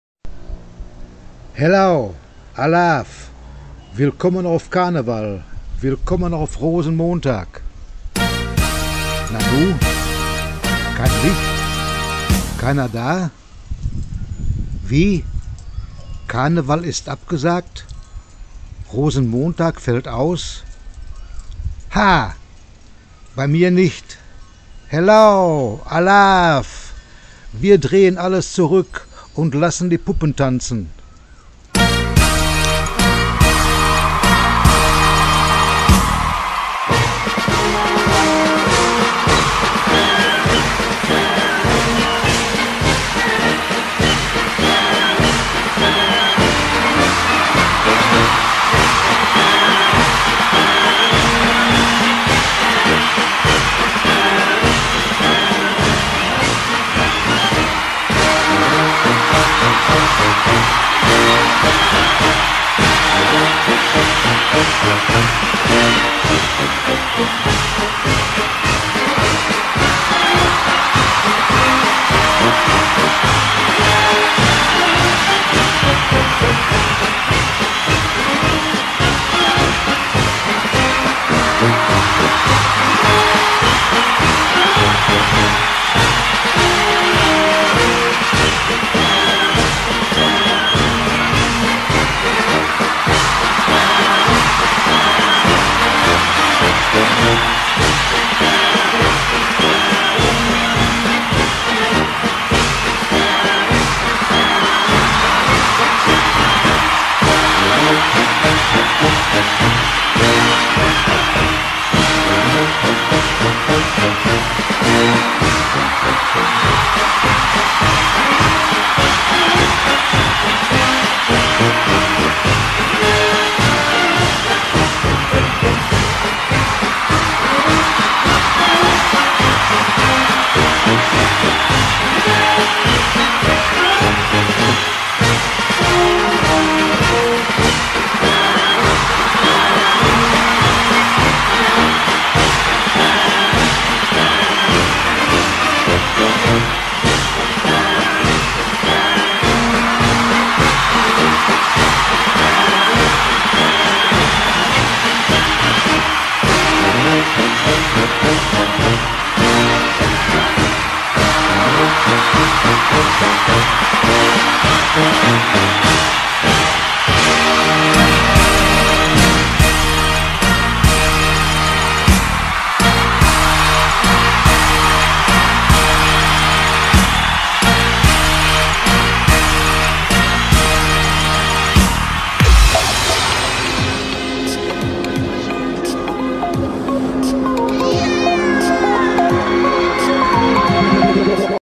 เกาะสมุย Koh Samui - Karneval, Rosenmontag, Tanzmariechen, GoGo-Girls, Corona, Covid-19, Helau
Vierzehn echte deutsche Frauen, keine Diversen, keine Bartträgerinnen, keine Kopftücher.

Haben Sie Urheberrechte an Teilen des zusammengestellten Materials? Dann bitte ich um ein nachsichtiges Lächeln und Würdigung meiner kreativen Leistung. Danke.